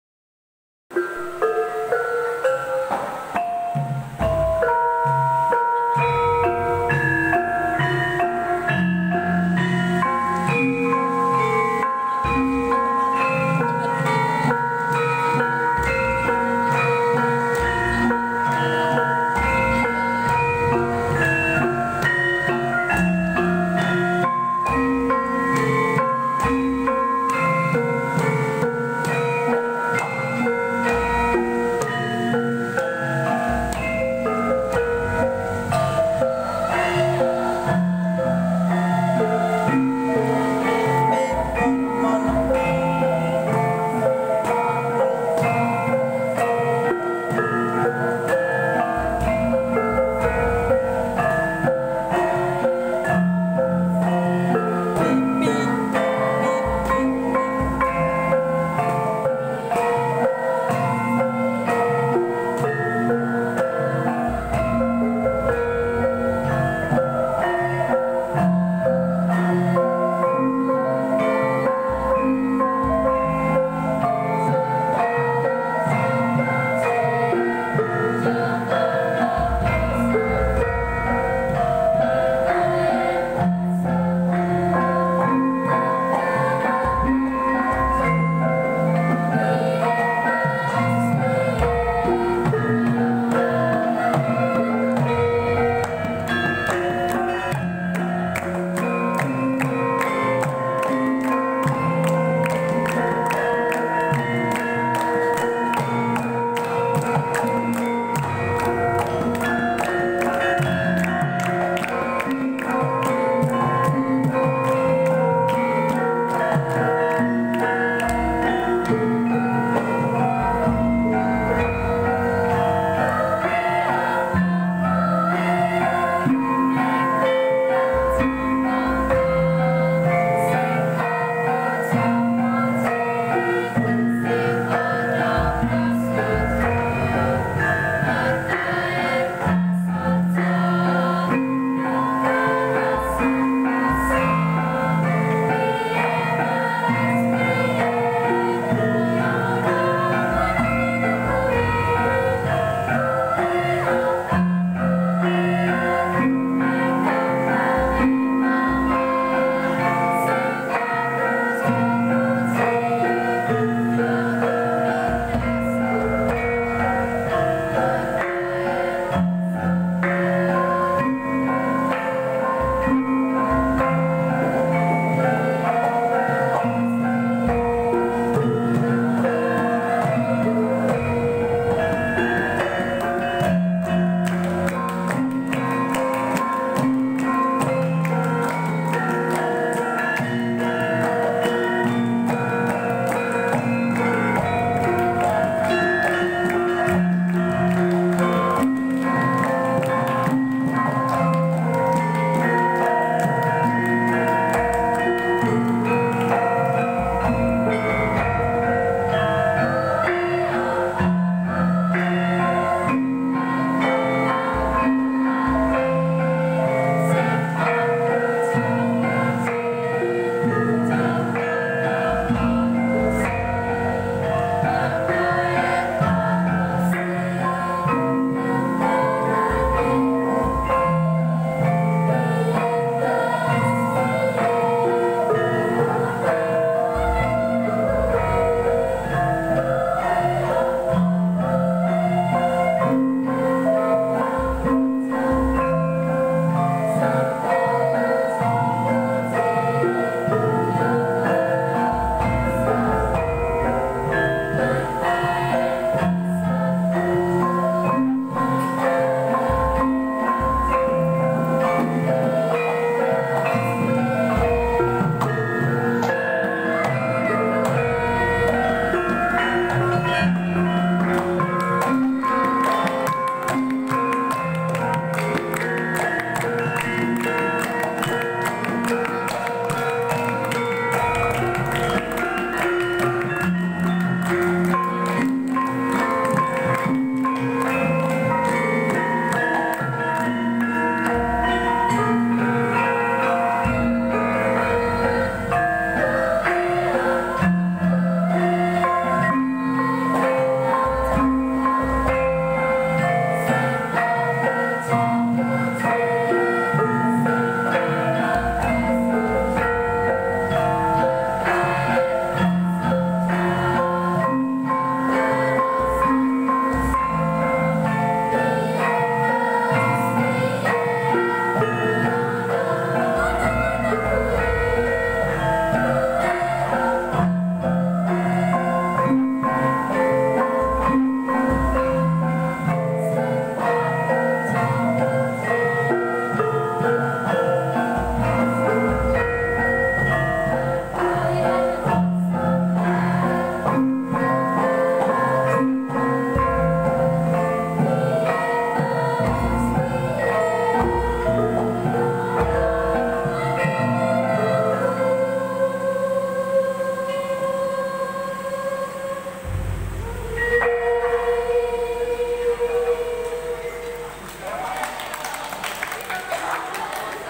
Metode pelatihan karawitan bersama mahasiswa ISI Surakarta di SMPN 4 Surakarta
ekstrakurikuler karawitan seni karawitan bersama mahasiswa ISI Surakarta jurusan karawitan dalam kegiatan metode pelatihan karawitan.